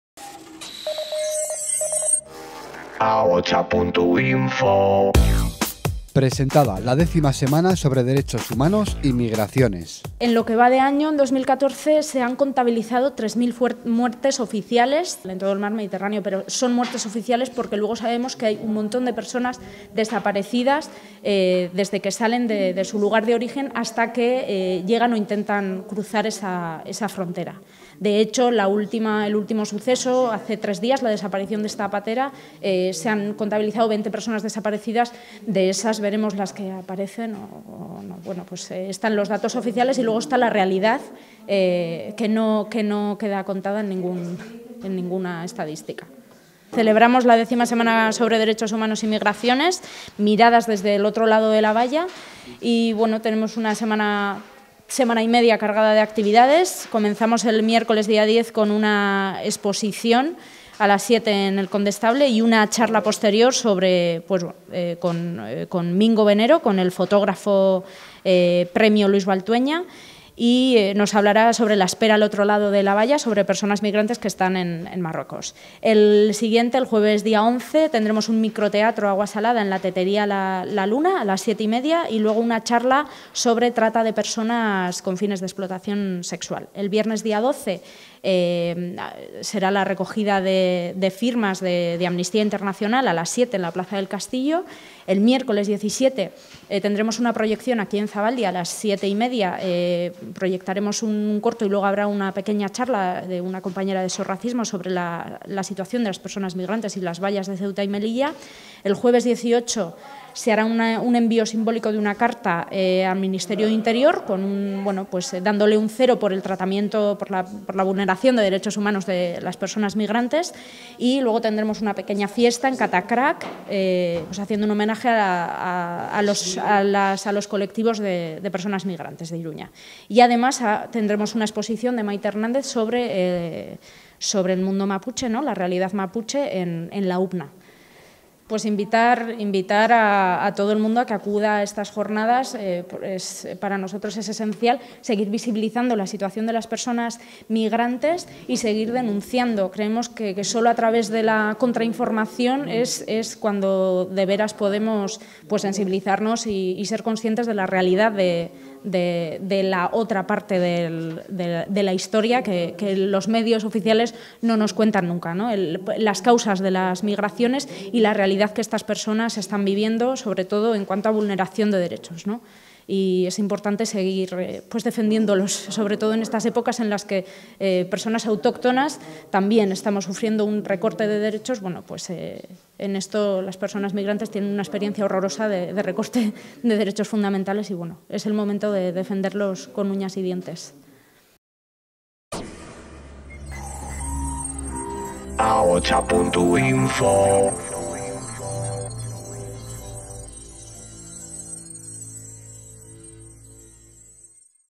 X Semana sobre Migraciones y DDHH
Presentación de la Semana de DDHH y Migraciones, donde Paperak Denontzat y Sos Arrazakeria han realizado una valoración de la actual situación en materia de derechos humanos de las personas migrantes.